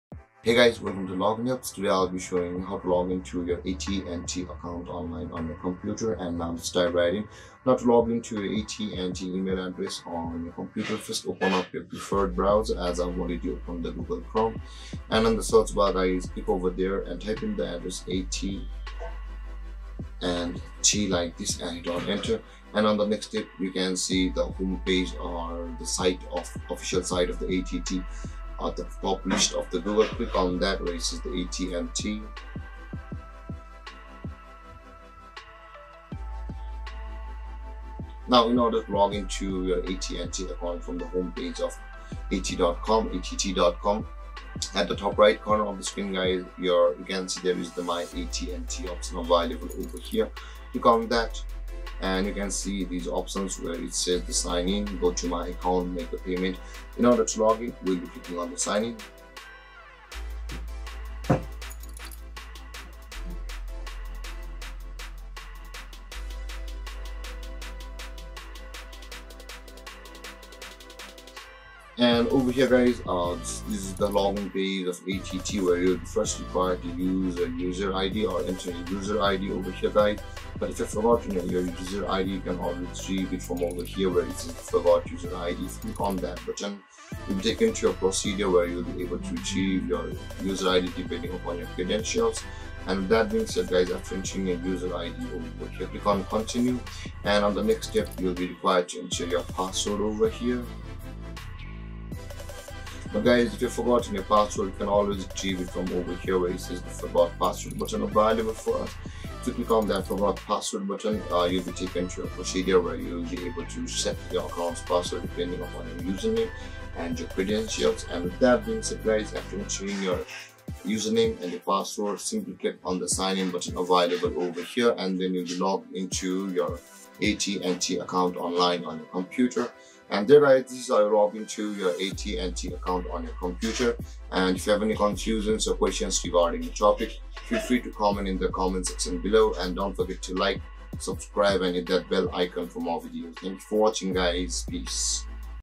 How to Login ATT account Online?
Welcome to our helpful guide on how to seamlessly access your AT&T account! Whether you're looking to manage your services, review your billing information, or explore your AT&T email, we've got you covered. In this video, we'll walk you through the step-by-step process of logging in to your AT&T account online. Learn how to effortlessly sign in at AT&T's official website or through the myAT&T app. No matter if it's checking your email or reviewing your account settings, we're here to make the login process smooth and hassle-free. Join us to unlock the full potential of your AT&T account today!